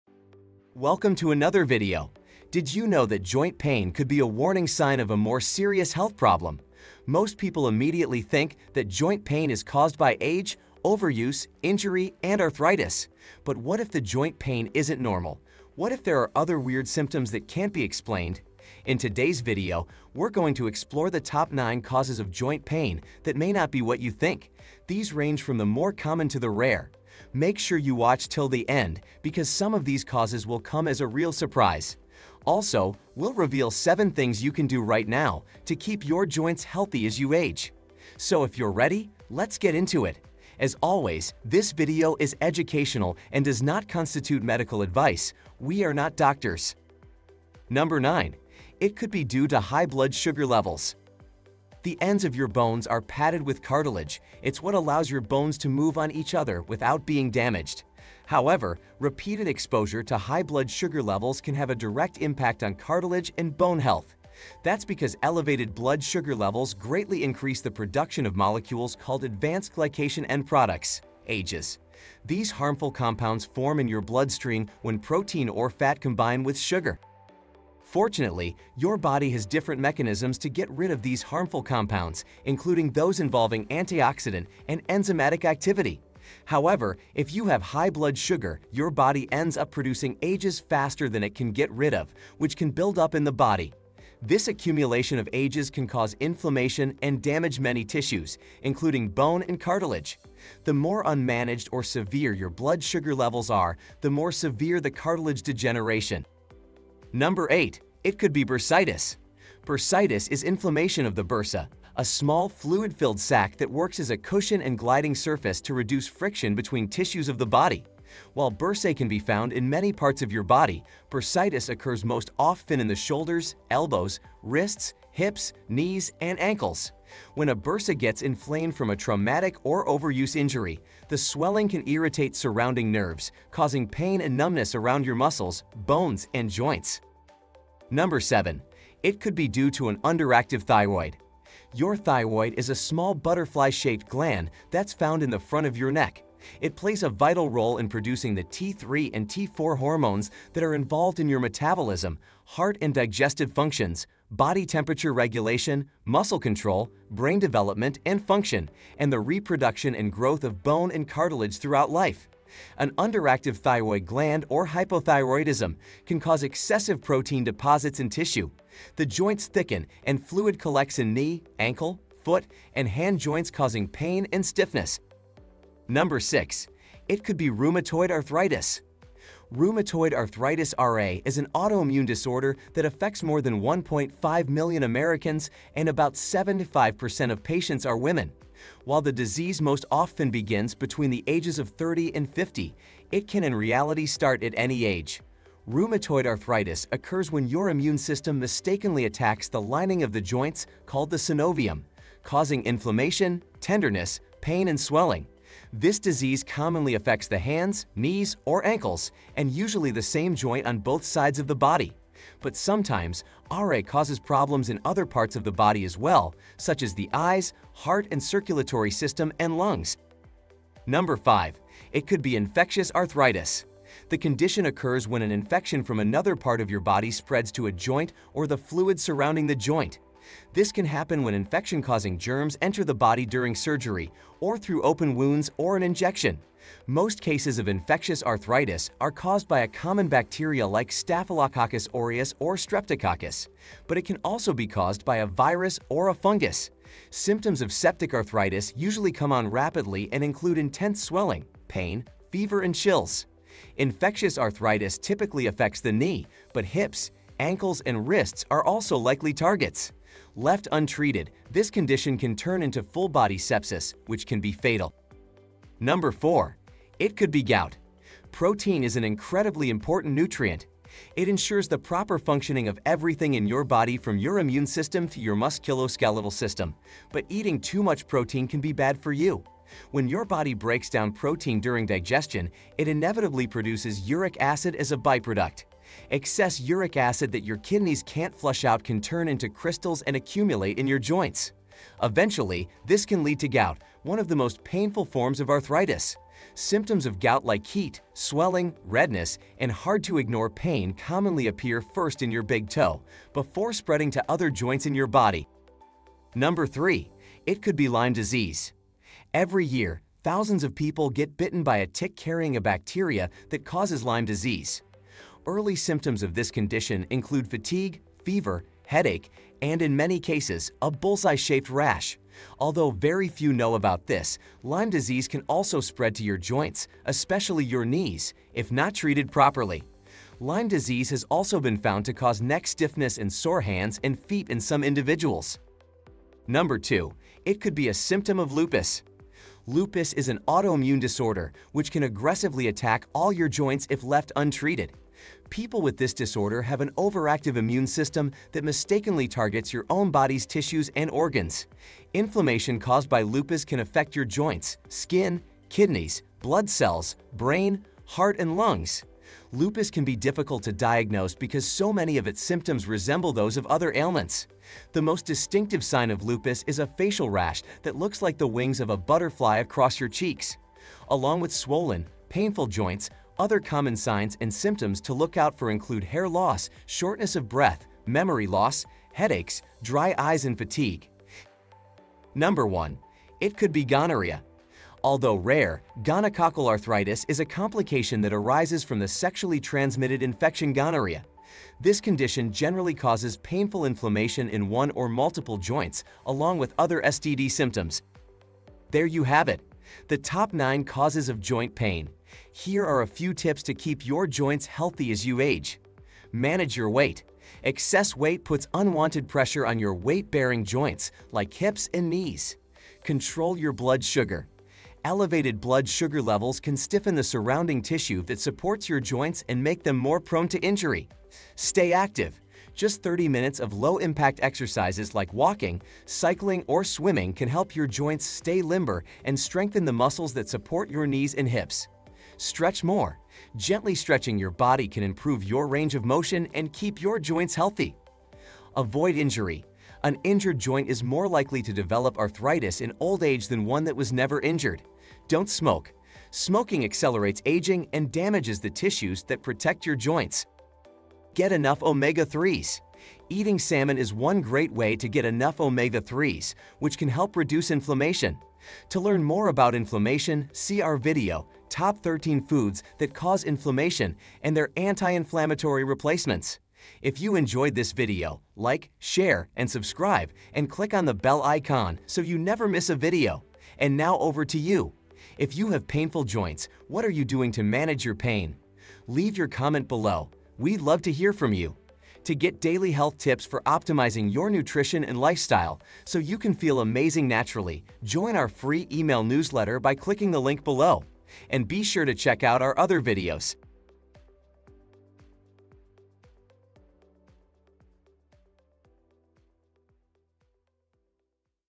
Top 9 Causes of Joint Pain
Discover the top 9 causes of joint pain.✅ Get your FREE Anti-Inflammatory Diet Plan ⏩

Summary:
Did you know that joint pain could be a warning sign of a more serious health problem? Most people immediately think, that joint pain is caused by age, overuse, injury, and arthritis. But what if the joint pain isn’t normal? What if there are other weird symptoms that can’t be explained? In today’s video, we’re going to explore the top 9 causes of joint pain that may not be what you think; make sure you watch till the end.

Timestamps for "Top 9 Causes of Joint Pain"

00:00 Intro
00:48 It could be due to high blood sugar levels.
01:47 It could be bursitis.
02:18 It could be due to an underactive thyroid.
02:56 It could be rheumatoid arthritis.
03:41 It could be infectious arthritis.
04:26 It could be gout.
05:07 It could be lyme disease.
05:37 It could be a symptom of lupus.
06:22 It could be Gonorrhea.
06:43 7 things you can do to keep your joints healthy

Did you enjoy our video on causes of joint pain? If you have arthritis or joint pain, how are you managing it?